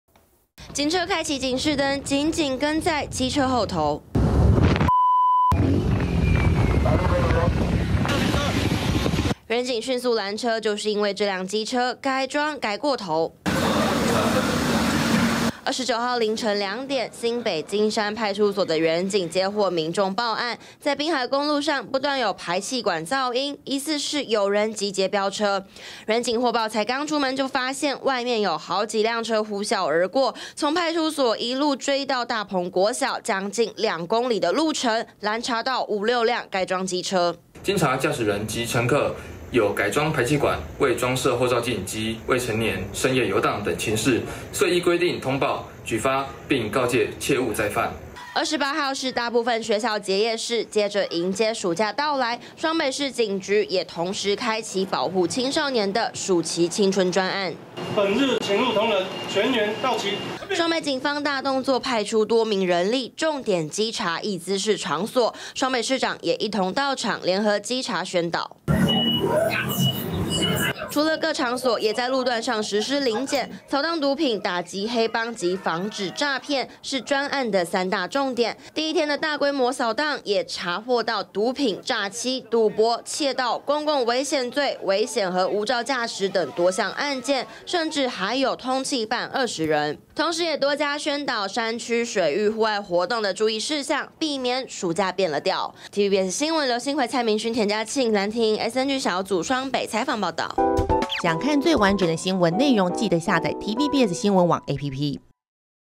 暑假第1天「開飆」 金山警攔改裝噪音機車｜TVBS新聞
學生迎來放暑假，才第一天在新北金山，就有員警攔查到5、6輛改裝機車在競速，其中還有未成年人士。而接下來兩個月，雙北市也開啟暑期青春專案，重點稽查各式場所，針對毒品、詐欺、黑幫等不法行為加強掃蕩。